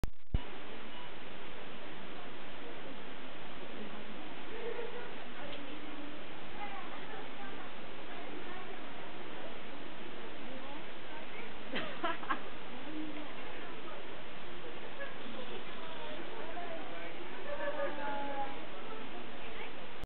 Take off your shoes....
A couple of kodomo (children) have their picture taken in wedding kimonos. (...and nifty hats) You can see everyone must remove their shoes before stepping inside on the floors lined with tatami mats.